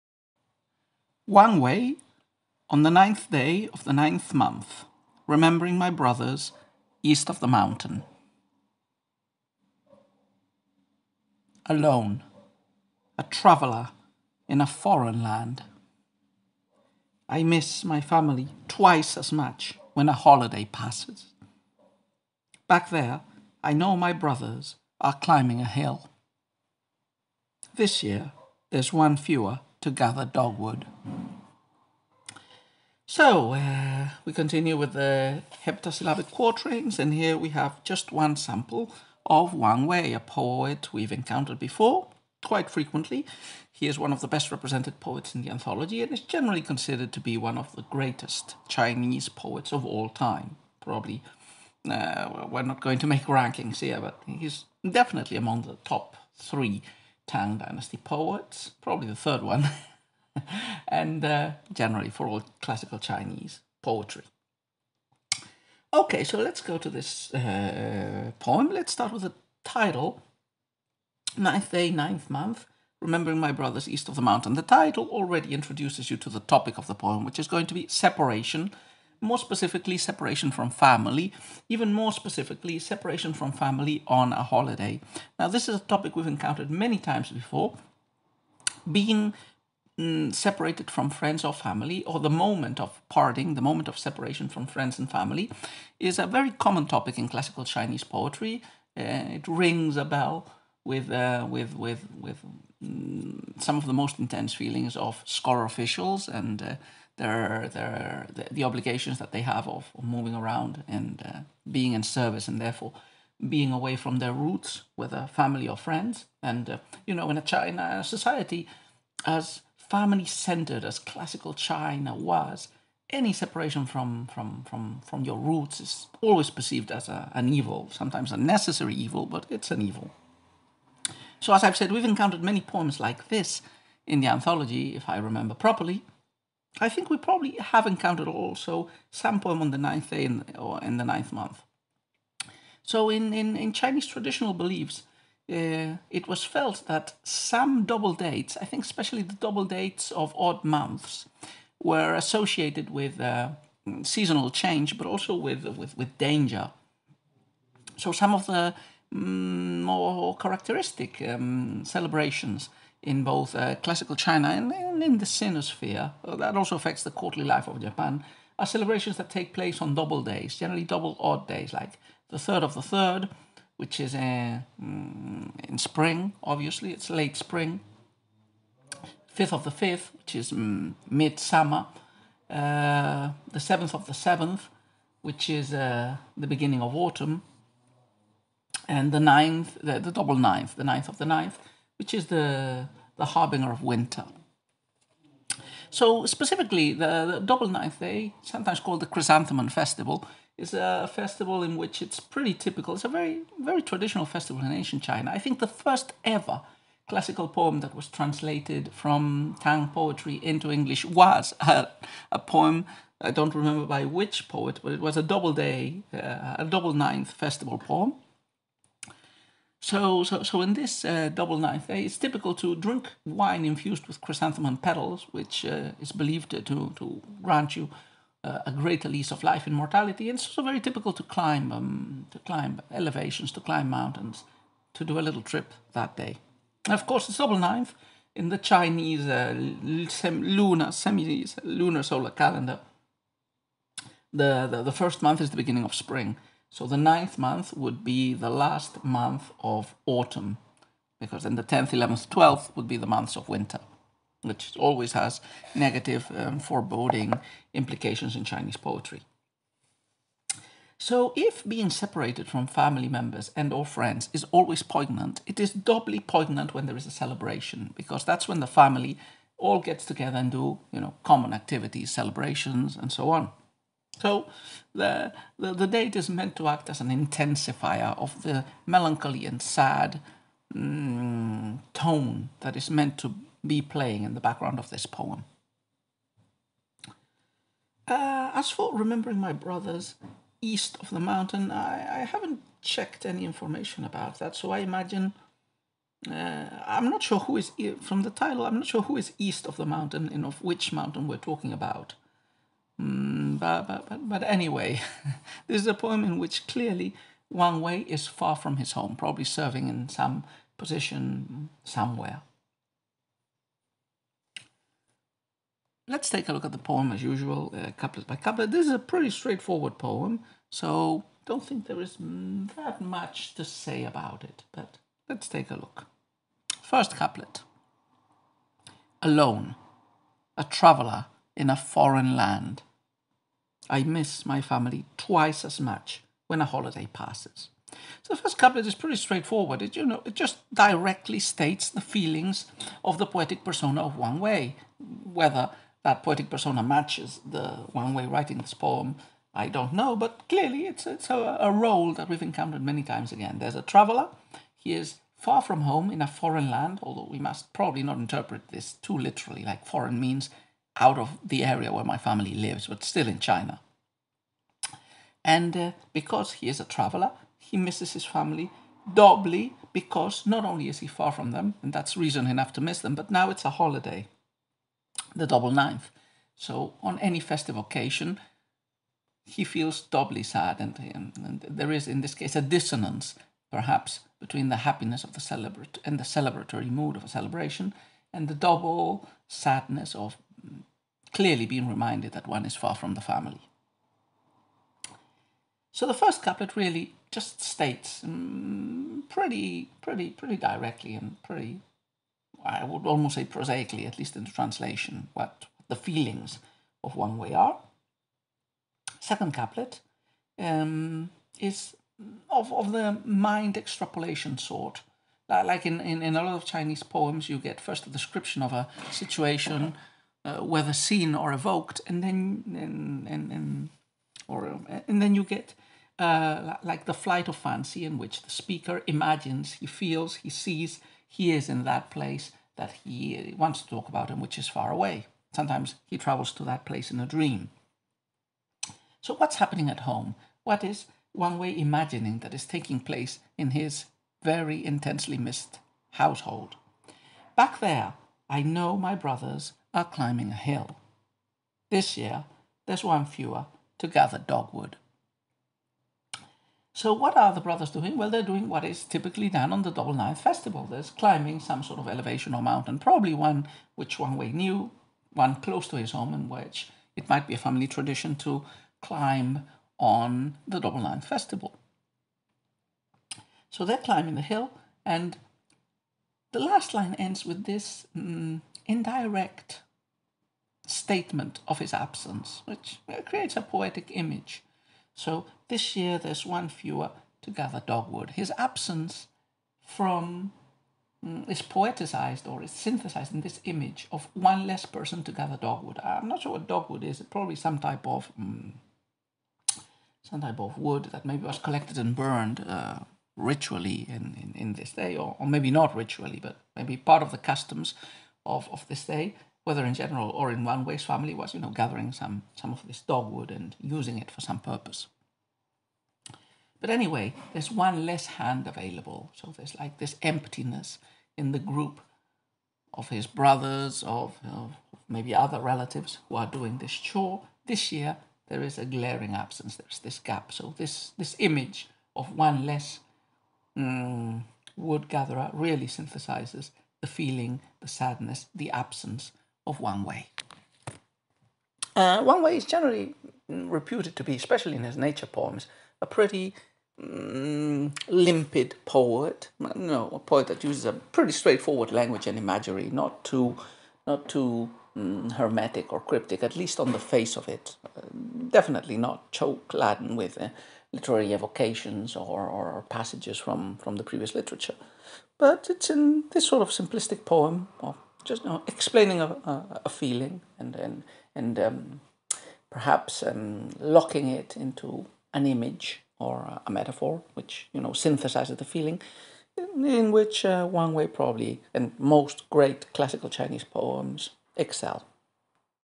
Wang Wei - On the 9th Day of the 9th Month, Remembering my Brothers
On celebration days, the poet feels lonelier than ever. "East of the mountain" seems to refer to Shandong, the eastern coastal province of northern China. "Dogwood" (Cornus Officinalis) branches were worn for good luck on the double ninth.